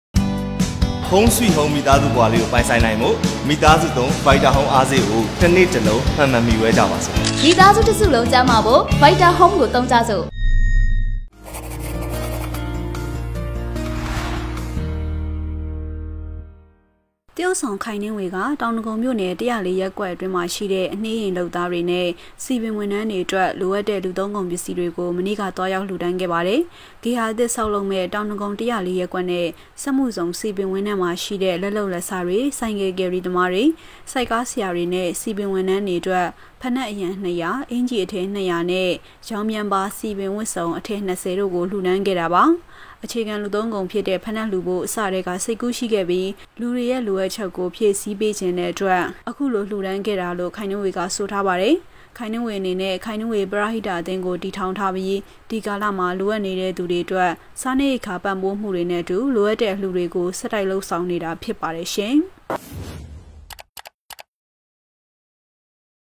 အနှေးယာဉ်လုပ်သားတွေနဲ့ စည်ပင်ဝန်ထမ်းတွေအတွက် ခိုင်နှင်းဝေရဲ့ အလှူ
ဇွန် - ၁၄၊၂၀၂၀

သရုပ်ဆောင်ခိုင်နှင်းဝေက တောင်ဒဂုံမြို့နယ် (၁၀၄)ရပ်ကွက်အတွင်းက အနှေးယာဉ်လုပ်သားတွေနဲ့ စည်ပင်ဝန်ထမ်းတွေအတွက် လိုအပ်တဲ့ လူသုံးကုန်ပစ္စည်းတွေကို မနေ့က သွားရောက်လှူဒါန်းခဲ့ပါတယ်။

ဂေဟာအသစ် ဆောက်လုပ်မယ့် တောင်ဒဂုံ (၁၀၄)ရပ်ကွက်နဲ့ စက်မှုဇုံစည်ပင်ဝင်းထဲမှာရှိတဲ့ လက်လုပ်လက်စားတွေ၊ ဆိုင်ကယ်ကယ်ရီတွေ၊ ဆိုက်ကားဆရာတွေနဲ့ စည်ပင်ဝန်ထမ်းတွေအတွက် ဖိနပ်အရံ ၂၀၀၊ အင်္ကျီအထည် ၂၀၀ နဲ့ ရောင်ပြန်ပါစည်ပင် ဝတ်စုံ အထည် ၂၀ တို့ကို လှူဒါန်းခဲ့တာပါ။

အခြေခံလူသုံးကုန်ဖြစ်တဲ့ ဖိနပ်လှူဖို့ အစကတည်းက စိတ်ကူးရှိခဲ့ပြီး လူတွေရဲ့လိုအပ်ချက်ကို ဖြည့်ဆည်းပေးချင်တဲ့အတွက် အခုလိုလှူဒါန်းခဲ့တာလို့ ခိုင်နှင်းဝေက ဆိုထားပါတယ်။

ခိုင်နှင်းဝေအနေနဲ့ ခိုင်နှင်းဝေပရဟိတအသင်းကိုတည်ထောင်ထားပြီး ဒီကာလမှာ လိုအပ်နေတဲ့သူတွေအတွက် စားနပ်ရိက္ခာပံ့ပိုးမှုတွေနဲ့အတူ လိုအပ်တဲ့အလှူတွေကိုဆက်တိုက်လုပ်ဆောင်နေတာဖြစ်ပါတယ်။

သတင်း - အေးမြတ်